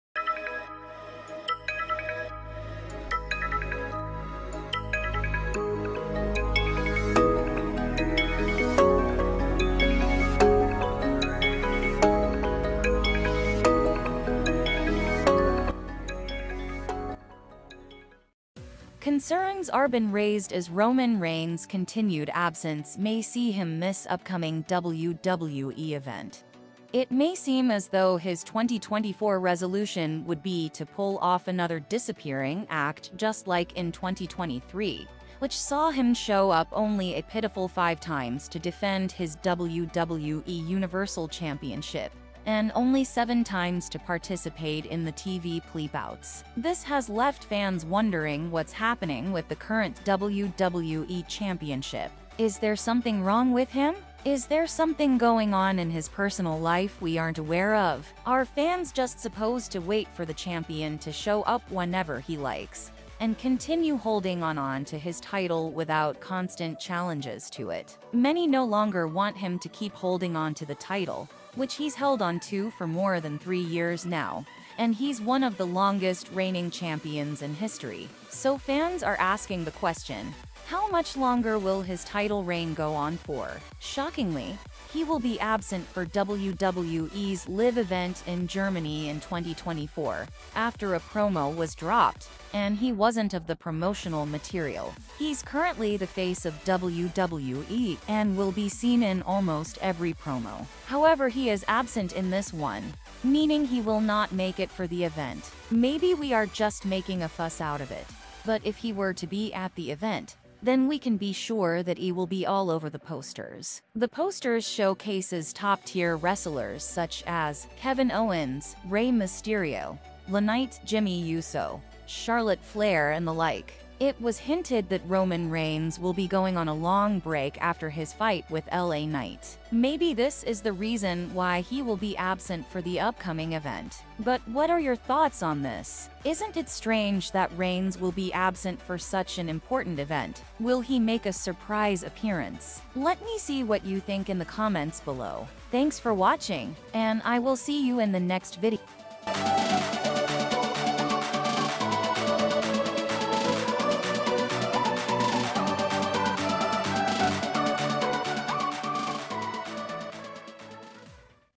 WHAT HAPPENED TO ROMAN REIGNS? Huge Concerns Raised As Roman Reigns Set To Miss Upcoming WWE Event
Roman Reigns will be absent for WWE's Live Event in 2024 in Germany. This has left fans concerned about what is going on with him. He isn't on the posters which very much confirms that he will not be there.